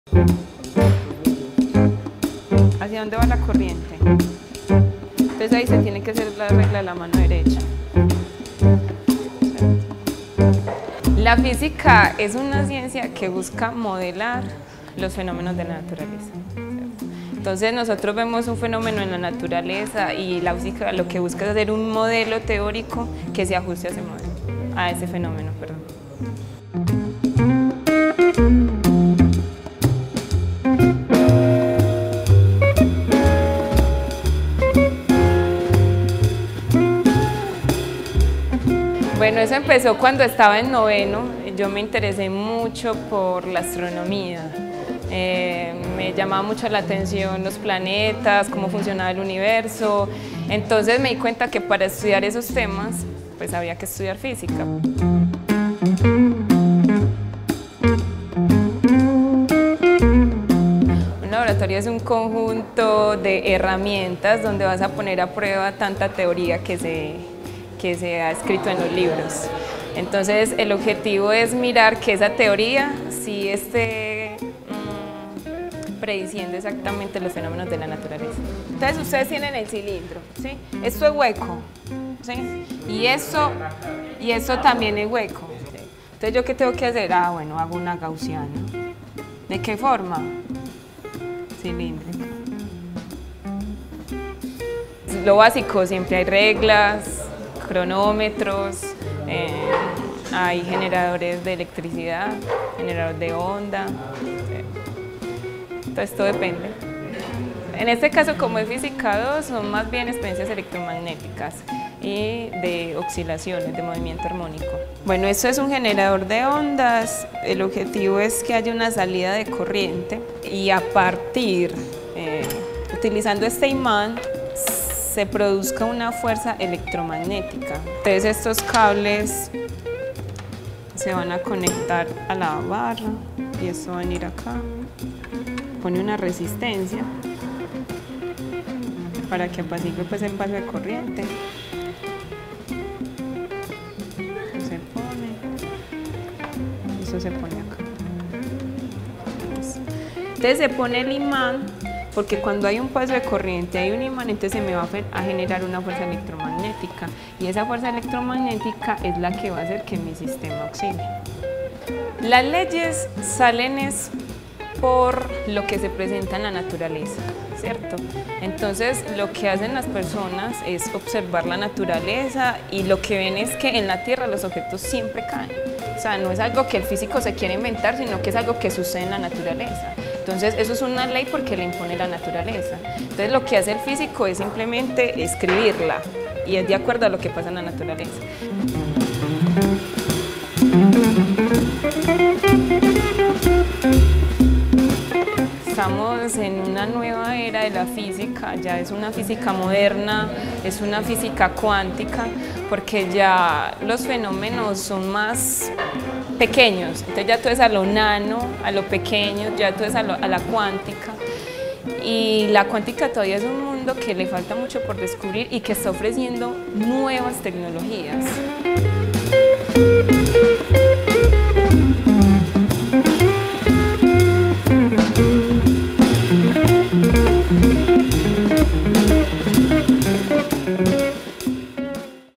Laboratorio de Física Universidad de Antioquia
La física es la ciencia natural de las propiedades del espacio, el movimiento, el tiempo, la materia, la energía y la masa así como sus interacciones...o sea de la vida!
Por eso nos metimos por un rotico a un laboratorio de física de una universidad, para que nos mostraran su espacio de trabajo, y los experimentos que estaban haciendo.